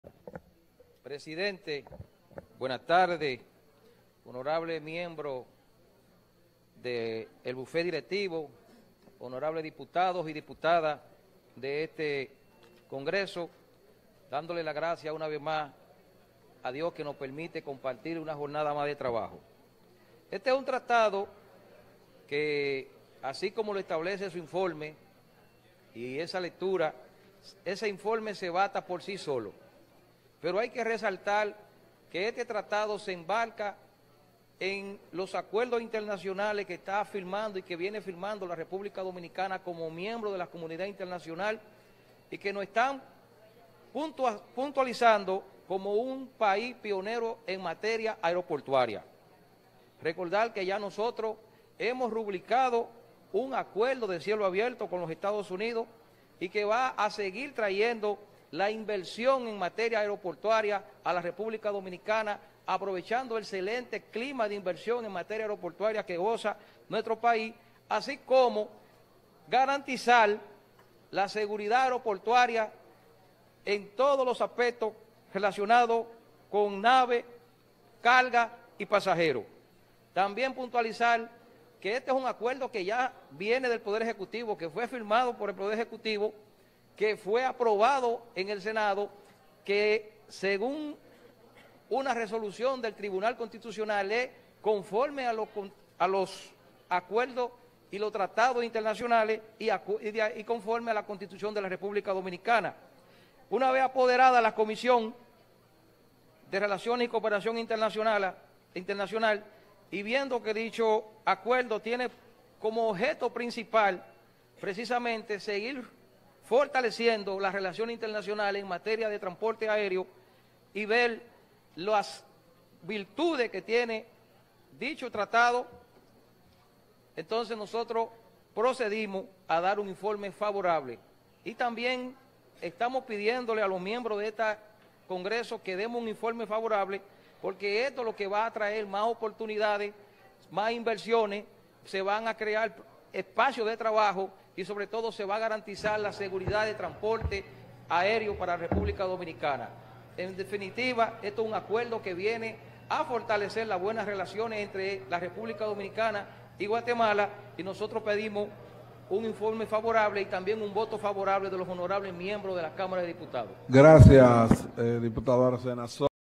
Turno previo del diputado Ignacio Aracena, Sesión No. 0017 miércoles 16 de octubre de 2024
El diputado Ignacio Aracena, como presidente de la Comisión Permanente de Relaciones Exteriores, expresa que el informe leído se basta por sí solo y destaca que puntualiza al país en un lugar importante en los acuerdos aéreos. Dice que busca fortalecer las relaciones comerciales entre la República Dominicana y Guatemala. Solicita el voto favorable.